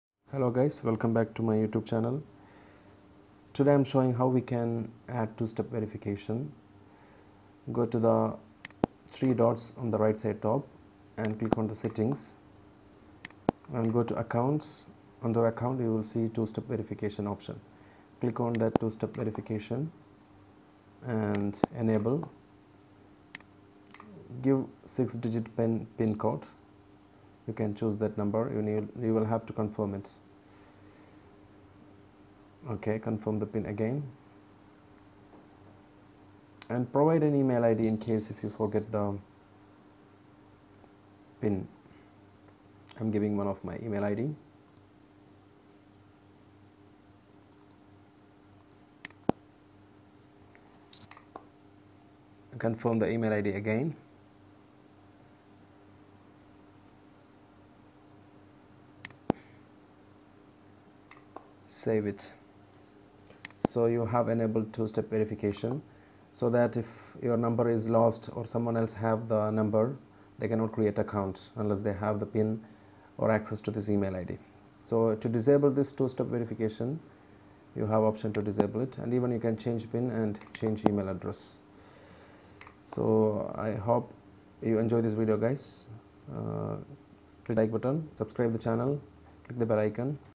Two-Step Verification in WhatsApp - How to Enable 2 Step verification in WhatsApp? [WhatsApp Tips]
In this video , I am showing how we can add security to our WhatsApp by adding two step verification.

Adding two-step verification is very good when there is chance to miss your number or phone.

Two-step verification is an optional feature that adds more security to your account. When you have two-step verification enabled, any attempt to verify your phone number on WhatsApp must be accompanied by the six-digit PIN that you created using this feature.